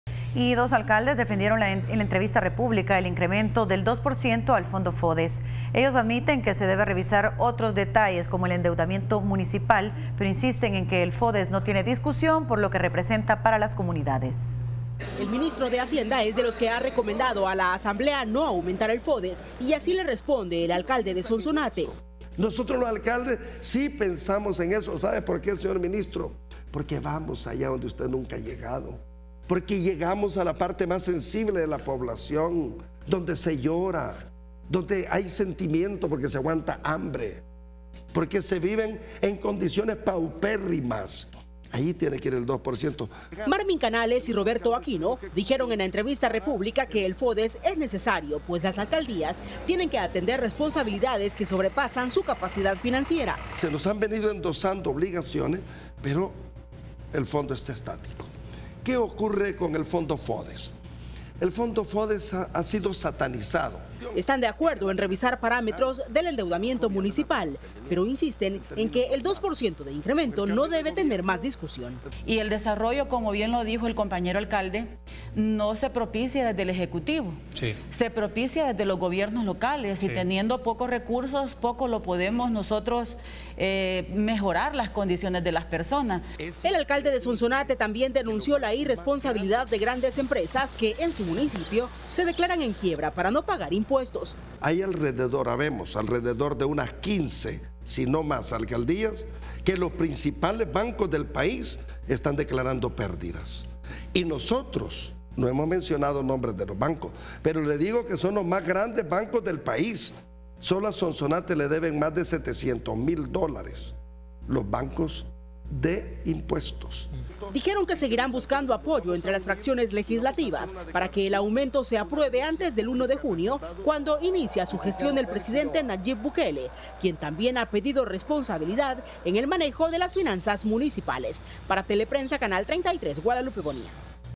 Alcaldes defienden el incremento al FODES
Dos alcaldes defendieron en la entrevista república el incremento del dos por ciento al fondo FODES.

Ellos admiten que se debe revisar otros detalles como el endeudamiento municipal, pero insisten en que el FODES no tiene discusión por lo que representa para las comunidades.